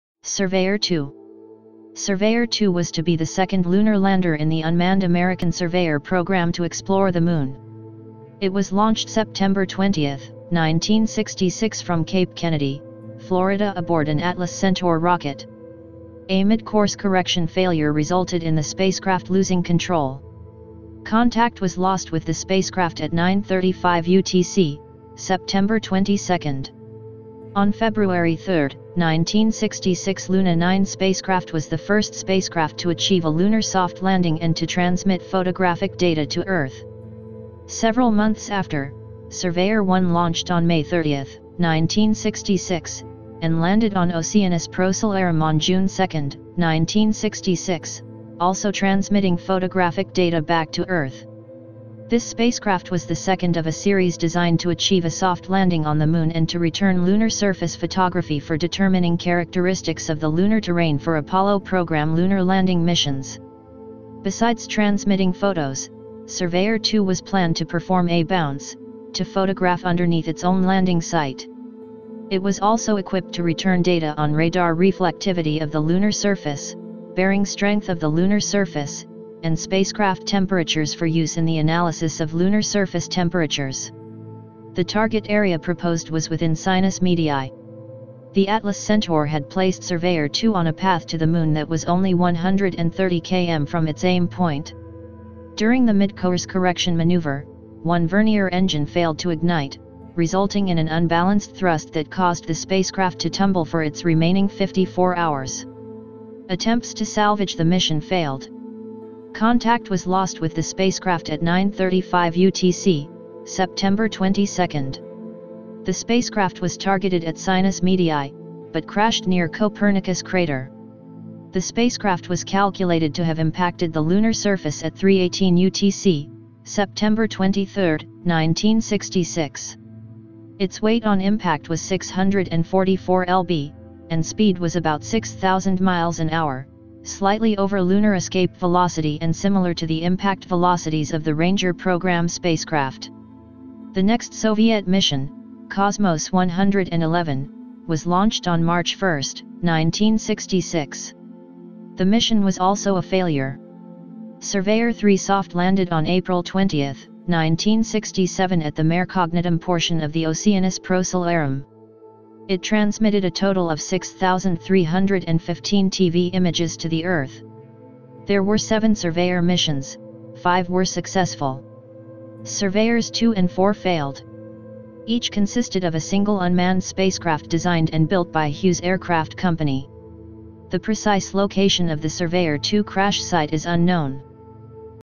Surveyor 2 🛰️🚀🌕 Our Stuff on the Moon 🌕🚀🛰️
Surveyor 2 was to be the second lunar lander in the uncrewed American Surveyor program to explore the Moon. It was launched September 20, 1966 from Cape Kennedy, Florida aboard an Atlas-Centaur rocket. A mid-course correction failure resulted in the spacecraft losing control. Contact was lost with the spacecraft at 9:35 UTC, September 22.
In this video series we'll cover all the artificial man-made objects that reside on the moon's surface.
This video is about "Surveyor 2".This video is about "Surveyor 2".